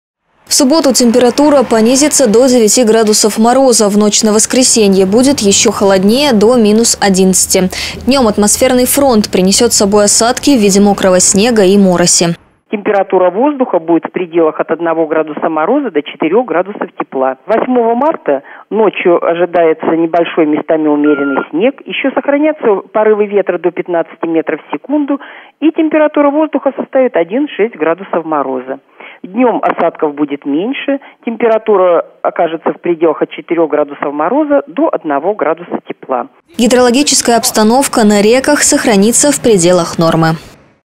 Какая погода ждет курян на 8 марта
Метеоновости на праздничные дни. По информации синоптиков в эти выходные курян ожидают колебания температуры и атмосферного давления. Прогнозируются порывы ветра и гололедица.